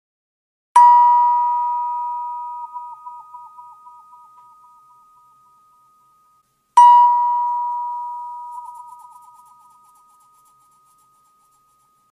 Wah Wah Tub mini
Wah wah tubo es una campana tubular en la que con solo el gesto de un dedo podemos modular el sonido. Se convierte en un instrumento ideal para la exploración de las propiedades sonoras de las campanas y los tubos sonoros.